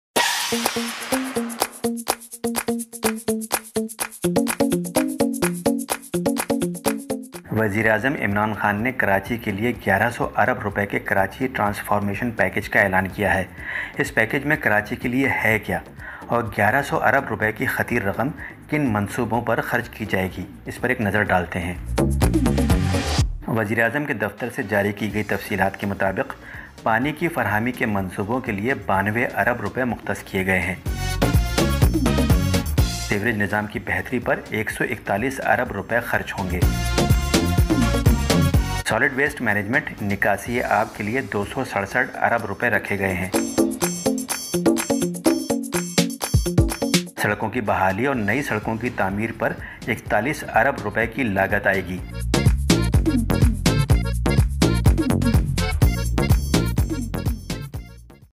What is included in Karachi Transformation Package | Khabar Kahani
What is included in Karachi Transformation Package

The Prime Minister's Karachi's package carries various projects for the city of Karachi. What are these projects, this news video elaborates those projects.

Earlier, the Prime Minister Imran Khan visited Karachi after the devastated torrential rains and announced that the federal government in collaboration with the provincial government will rebuild the city of Karachi. For this purpose, the Prime Minister has announced development work of Rs 1100 billion in different sectors including solid waste management, garbage lifting, construction of roads and cleaning of storm water drains.

You may also visit